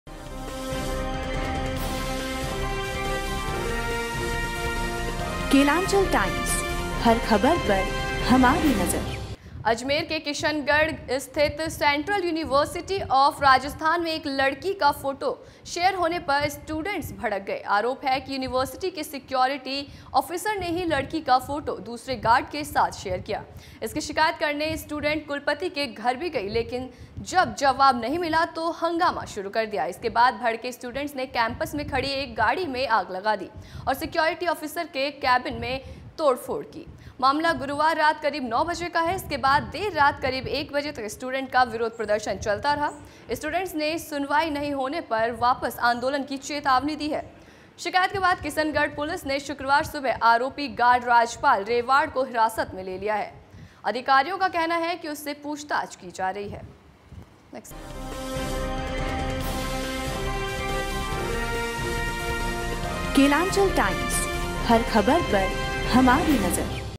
अजमेर में सेंट्रल यूनिवर्सिटी में हंगामा
अजमेर के किशनगढ़ स्थित सेंट्रल यूनिवर्सिटी ऑफ राजस्थान में एक लड़की का फोटो शेयर होने पर स्टूडेंट्स भड़क गए। आरोप है कि यूनिवर्सिटी के सिक्योरिटी ऑफिसर ने ही लड़की का फोटो दूसरे गार्ड के साथ शेयर किया।